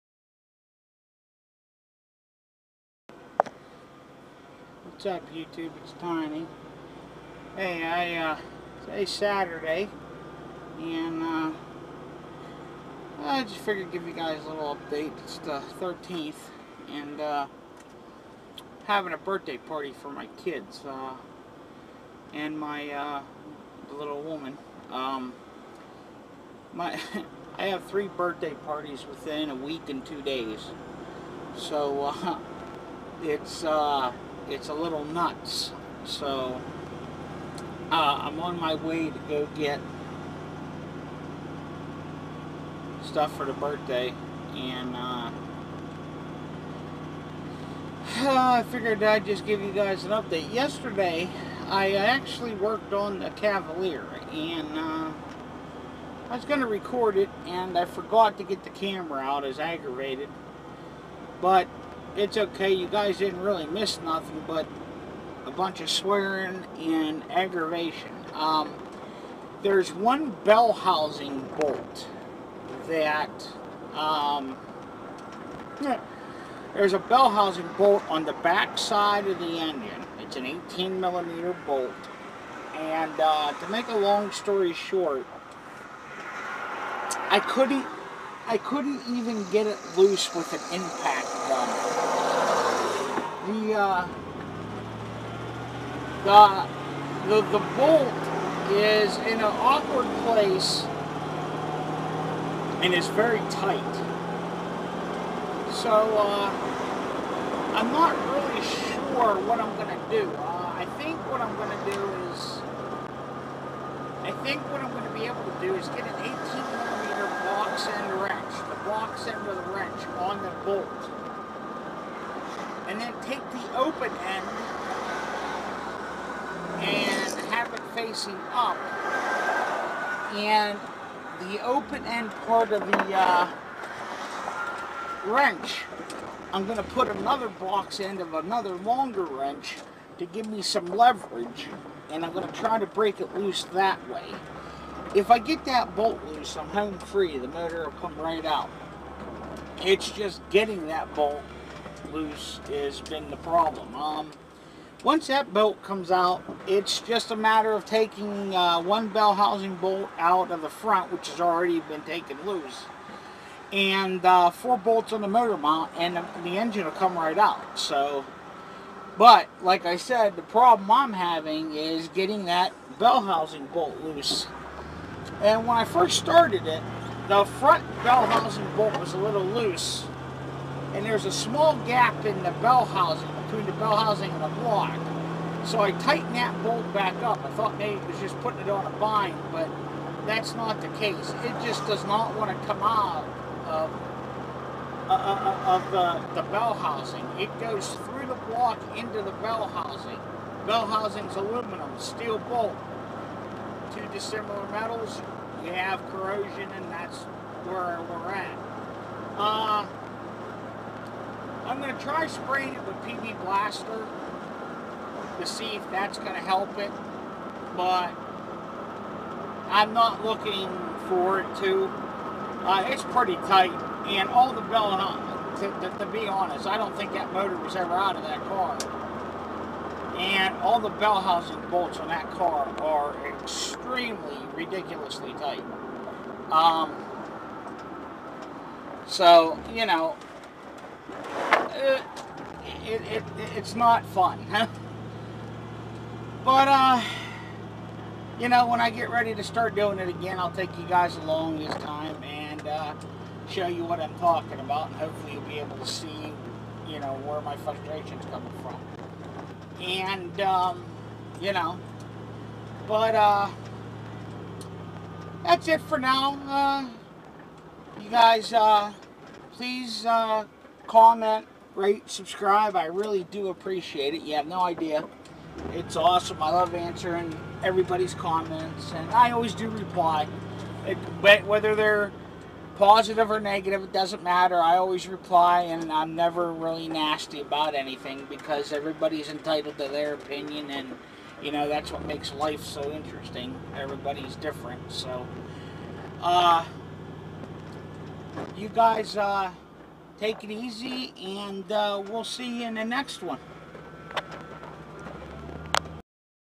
Frustration Vlog
Just venting bout problems I ran into workin on the caviler.Hope you like the video.I made it look like an old movie just to be different.